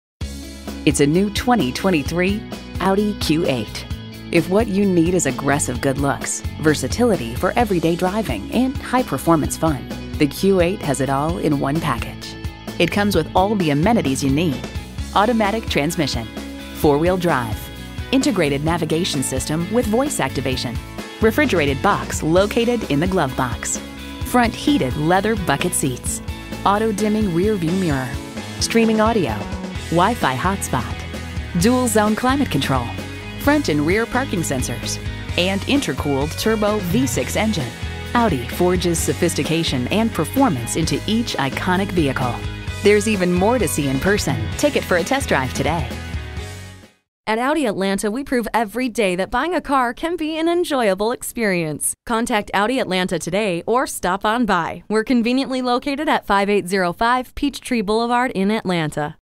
New 2023 Audi Q8 Atlanta Alpharetta, GA A23577 - SOLD
Year: 2023
Make: Audi
Model: Q8
Engine: 3.0L V6 Turbocharged DOHC 24V LEV3-ULEV70 335hp
Transmission: 8-Speed Automatic with Tiptronic
Color: White
Interior: Black
Stock #: A23577
VIN: WA1AVBF15PD021327

Address:
5805 Peachtree Boulevard